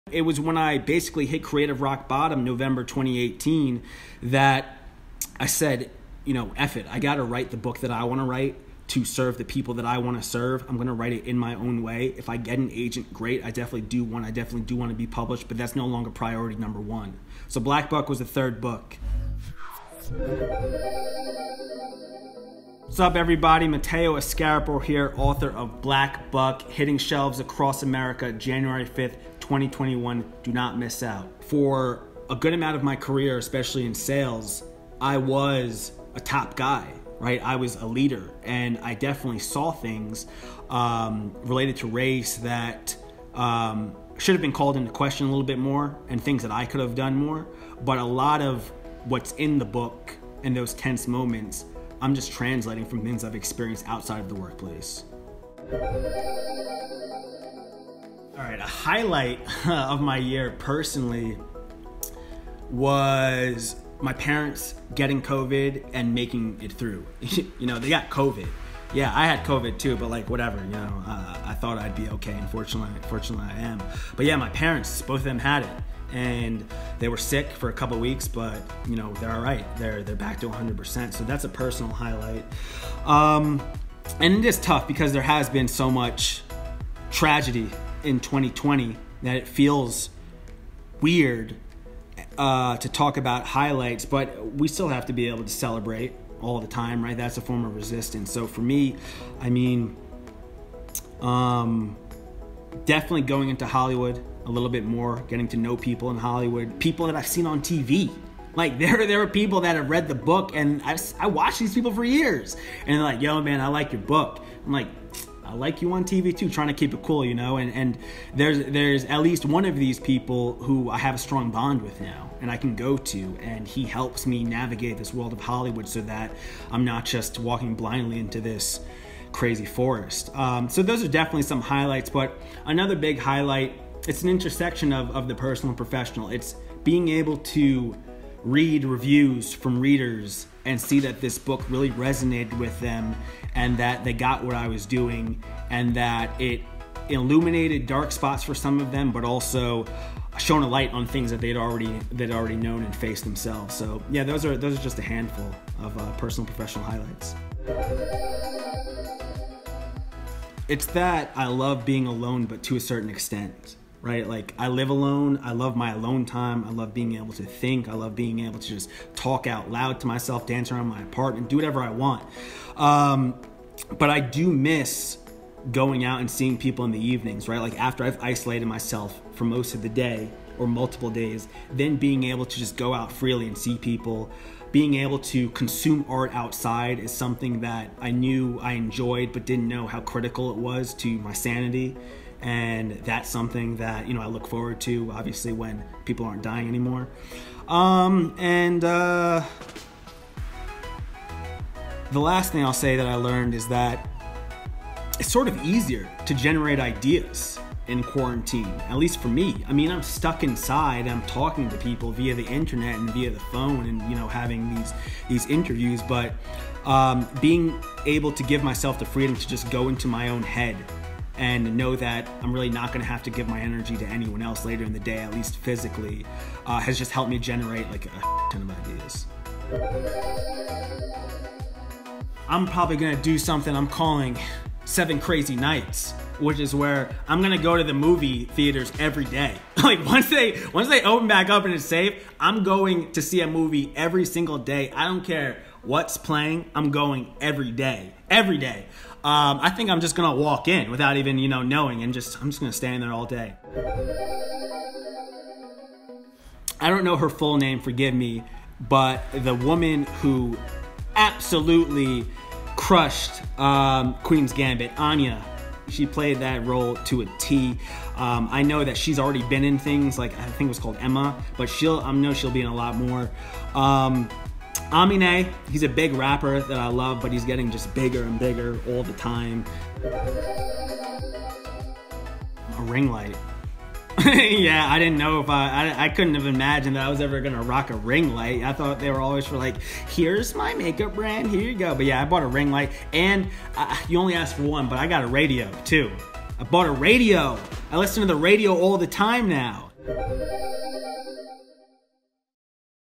Mateo Askaripour Opens Up About Debut Novel 'Black Buck' | Breaking Big | Entertainment Weekly
After pivoting to publishing fresh off a successful tech-sales career, Mateo Askaripour's debut novel, Black Buck — about a Black man thrust into the startup world — nabbed him a six-figure book deal. It's now a New York Times best-seller.